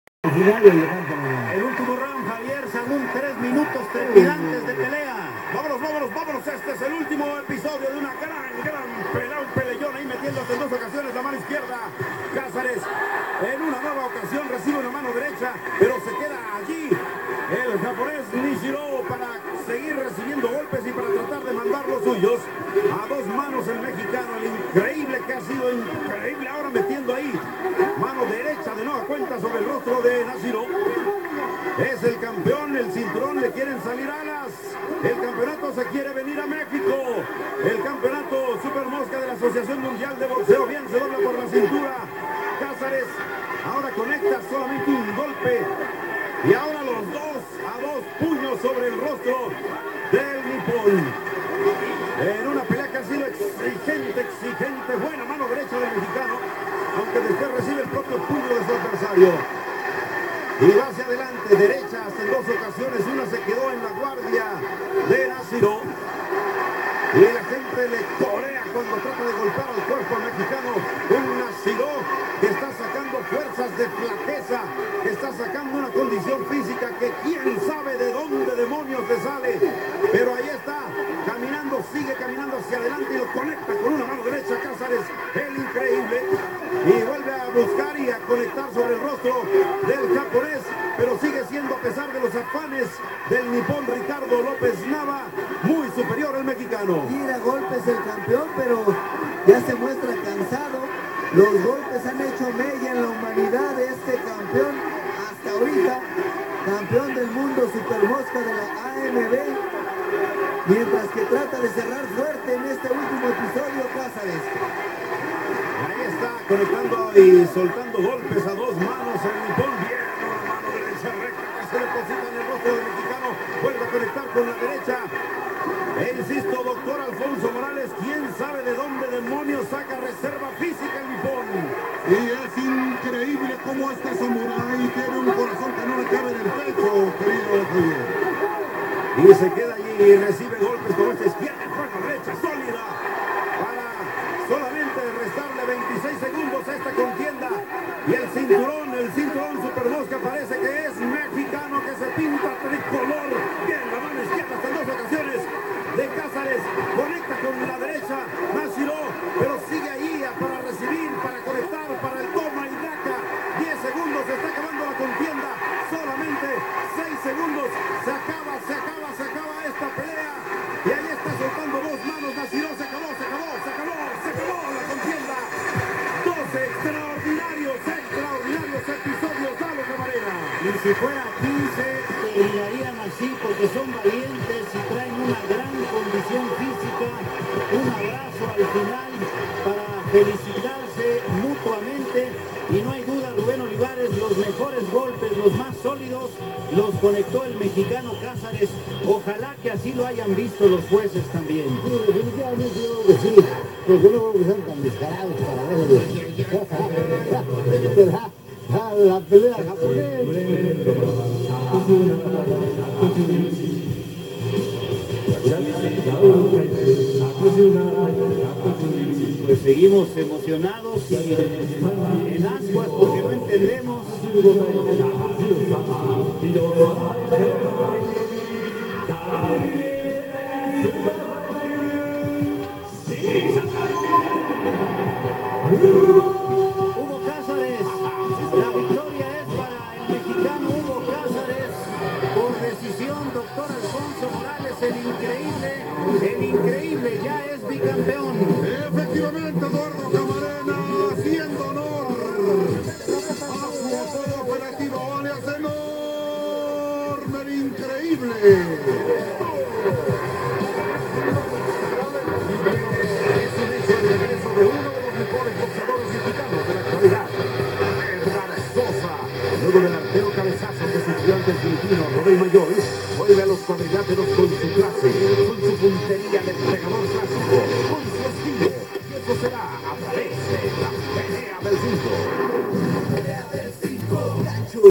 Hugo el Increible Cazares vs Nobou Nashiro Round 12.mp4
Una excelente pelea, hay que reconocer la condicion física del mexicano y del japones para llegar hasta el Round 12 peleando de tal manera... Felicitaciones para el Increible Cazares.